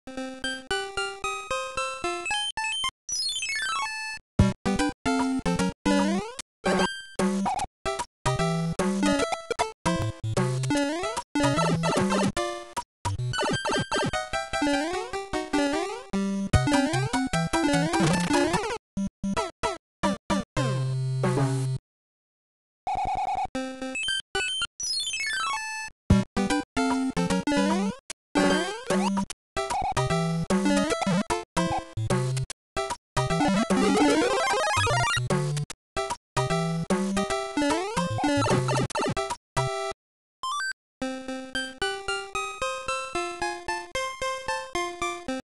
Reuben Bugs for Dahrk Daiz 12/17/2009
This is for DahrkDaiz. -The pointer works as it should if Mario is little, but it clears the level if he is powered-up -The Koopa Troopa moves right, when it should move left/ towards Mario